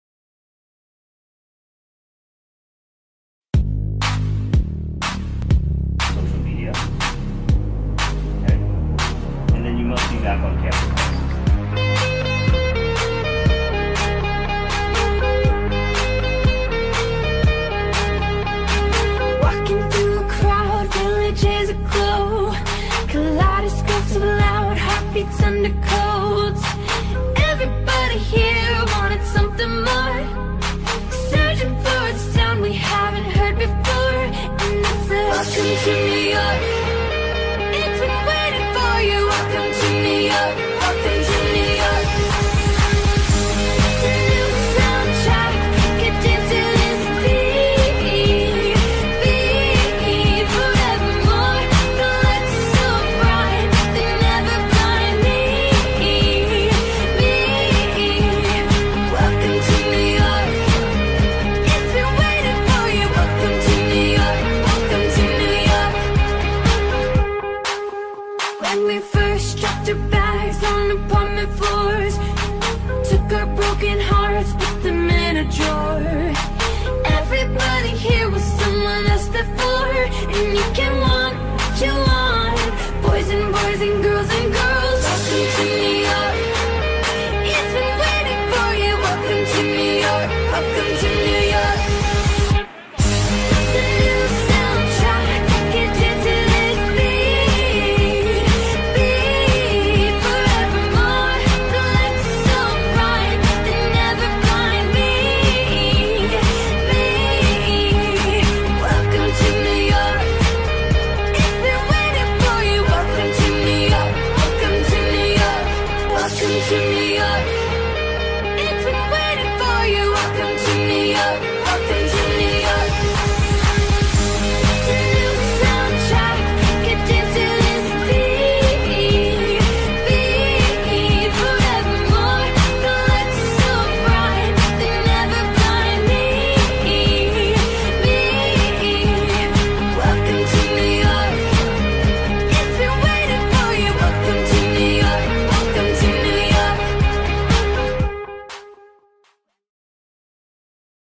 A Day in NYC: LIU Volleyball Scavenger Hunt
The LIU Brooklyn Volleyball team used their preseason off day to explore the Big Apple. Players were split into two teams and competed in a scavenger hunt throughout all of Brooklyn and Manhattan.